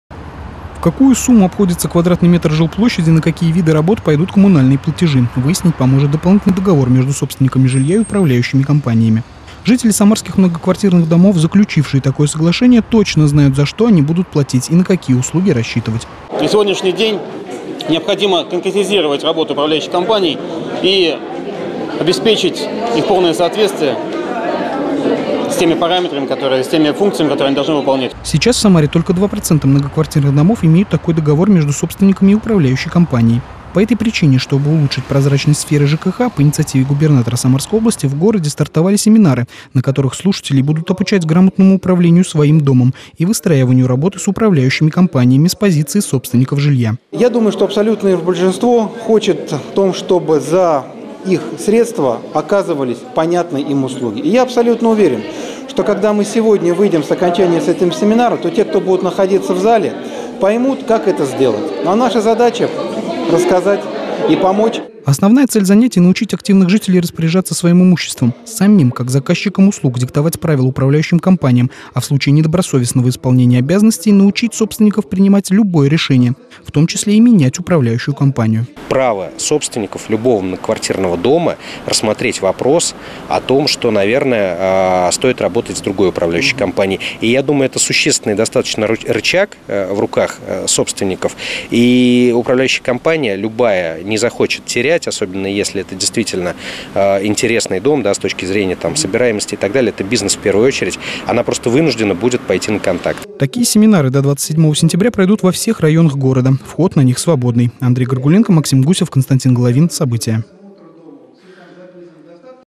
В Самаре проводят семинары - как правильно выстроить отношения с управляющей компанией
Управдомов тоже посадят за парты. Только не для уравнений и диктантов, а чтобы научить грамотному распоряжению своими деньгами. Правительство Самарской области проводят семинары, на которых рассказывают как правильно выстроить отношения с управляющей компанией, чтобы потом не расхлебывать коммунальные беды.